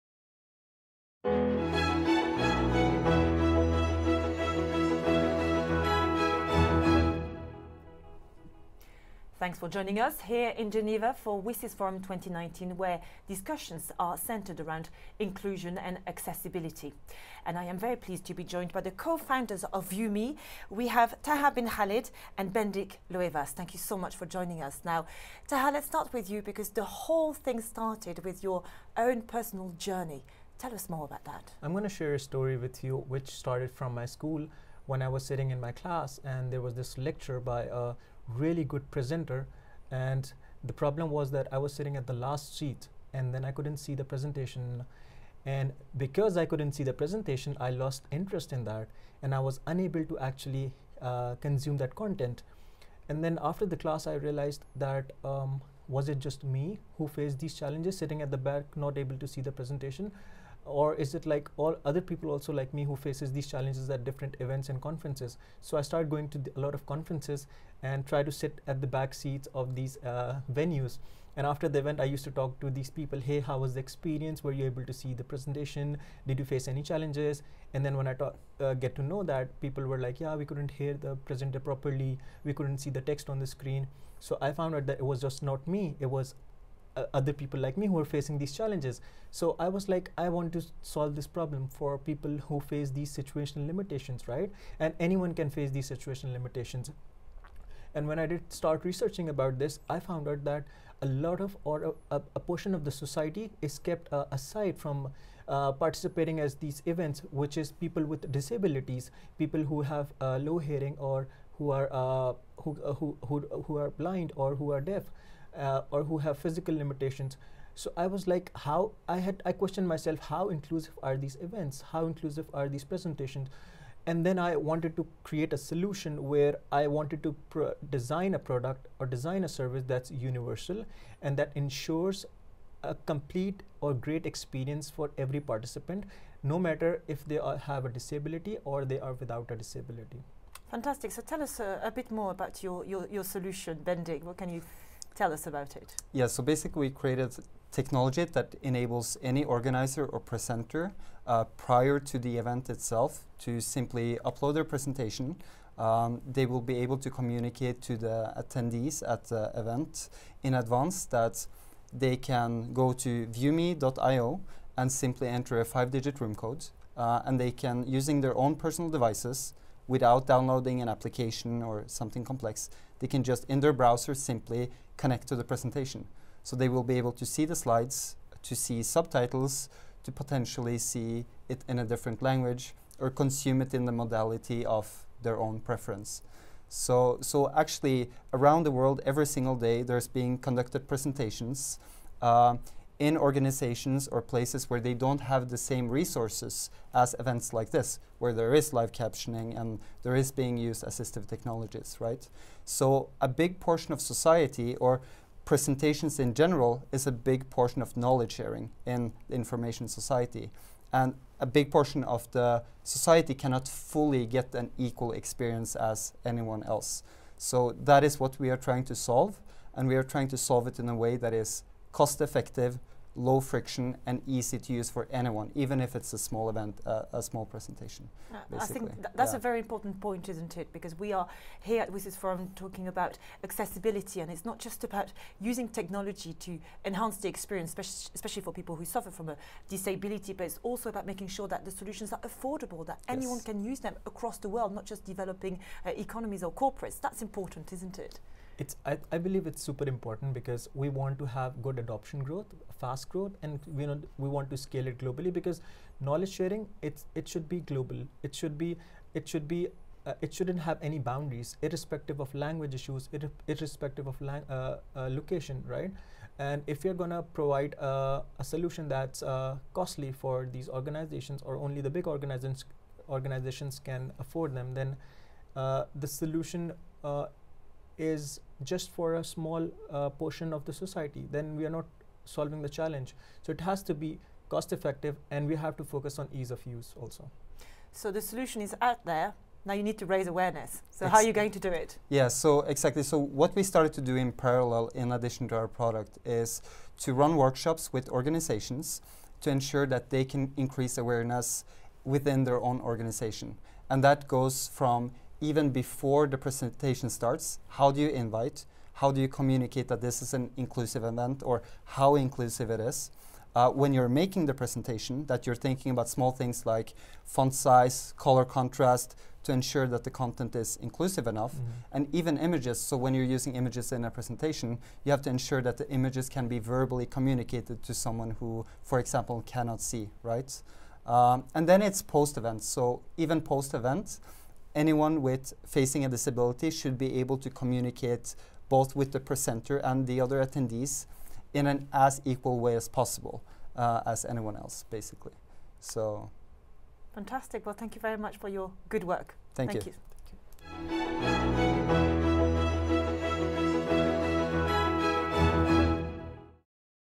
ITU WSIS FORUM 2019 INTERVIEWS: TAAHA BIN KHALID & BENDIK LOEVAAS, VUME, INCLUSIVE PRESENTATIONS
Interview with Taaha Bin Khalid and Bendik Loevaas, Founder and Co-founder, VUME, Inclusive Presentations at the WSIS FORUM 2019, ITU, Geneva, Switzerland.